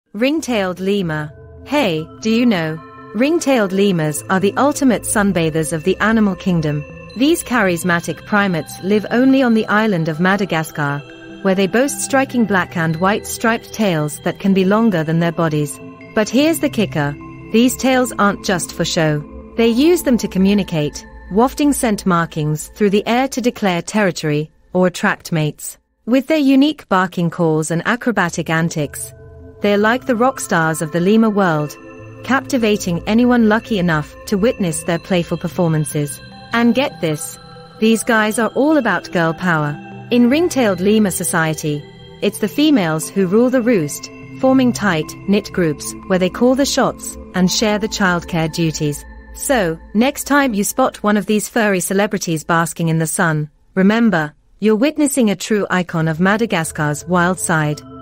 🌟 The Remarkable Story of the Ring-Tailed Lemur 🐾
Meet the captivating ring-tailed lemur! 🌿 These unique primates are native to the vibrant island of Madagascar 🇲🇬, boasting stunning black-and-white striped tails that serve as their signature feature. LemurLove MadagascarMagic 🌴 But there's more to these fuzzy celebrities than meets the eye! They're not just adorable, they're also incredible communicators, using their tails to send messages and mark territory. 📣 With their acrobatic skills and playful antics, they're the true stars of the jungle! 🌟 Plus, did you know? In their matriarchal society, female lemurs rule the roost, demonstrating girl power at its finest! 💪 Get ready to embark on an adventure into the wild world of the ring-tailed lemur! 🌟🐾